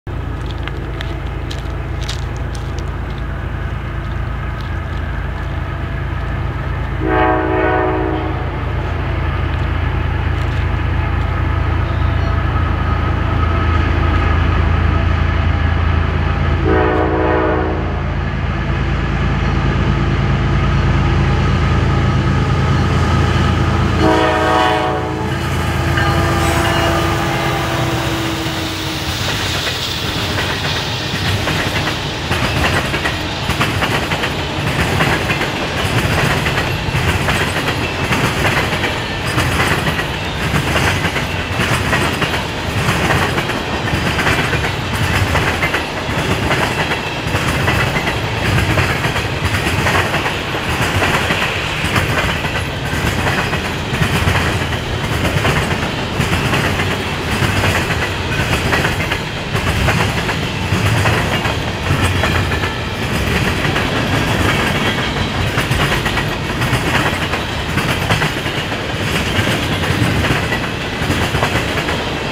KCS GE and BNSF SD70MACe lead CP 281!
281 knocks down the searchlight still standing guard at Lake Siding on the CP C&M Subdivision